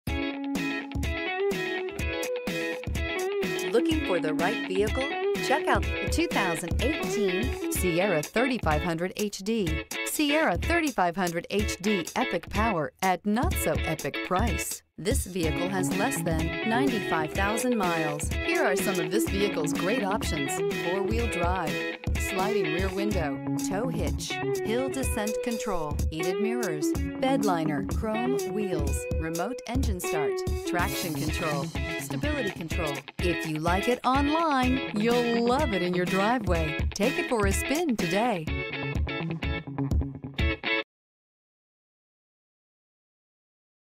2018 GMC Sierra 3500HD H005B
2018 GMC Sierra 3500HD Denali

2018 GMC Sierra 3500HD Denali Allison 1000 6-Speed Automatic, 4WD, Jet Black w/Perforated Leather Appointed Seat Trim, Driver Alert Package, Forward Collision Alert, Hill Descent Control, Lane Departure Warning System, Navigation System, Off-Road Suspension Package. Denali Priced below KBB Fair Purchase Price!